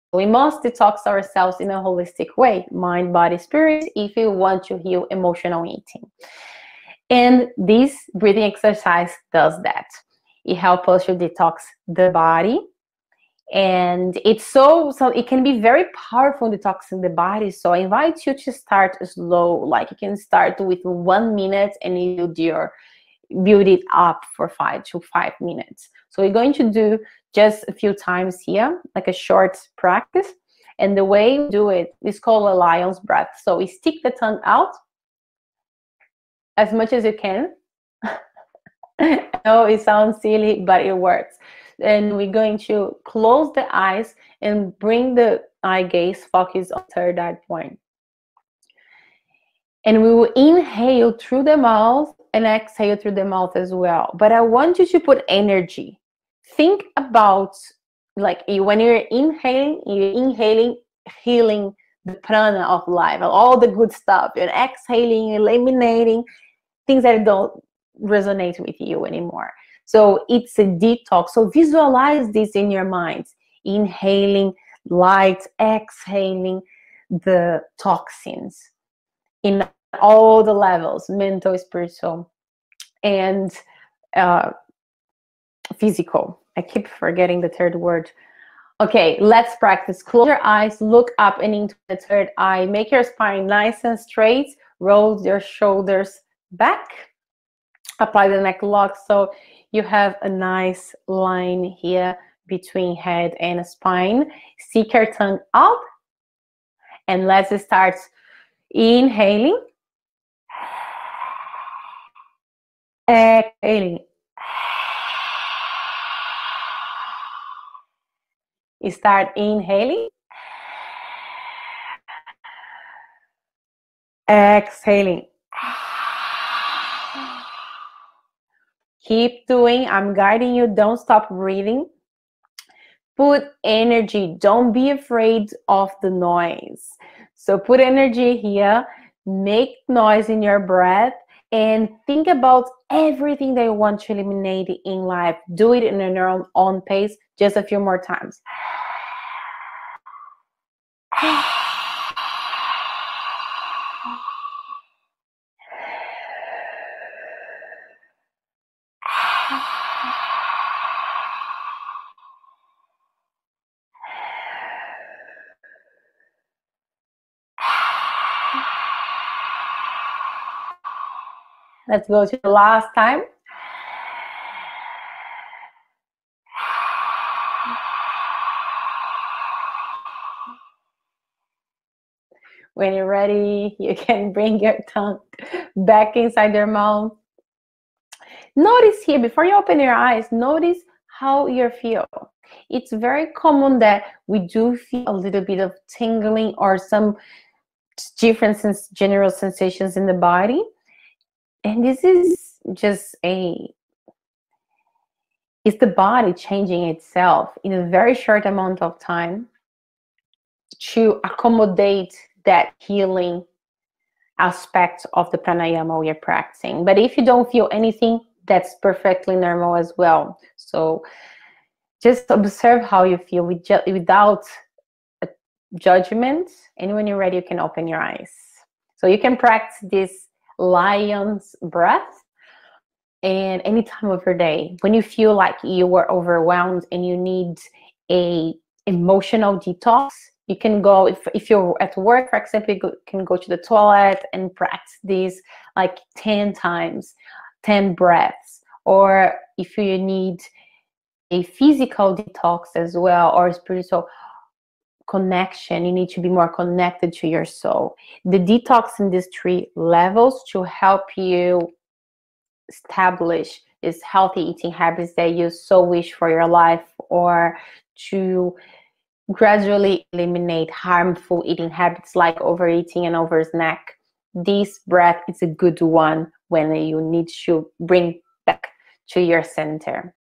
Detox bad eating habits with this pranayama.
We must detox ourselves in a holistic way: mind-body-spirit if we want to heal emotional eating.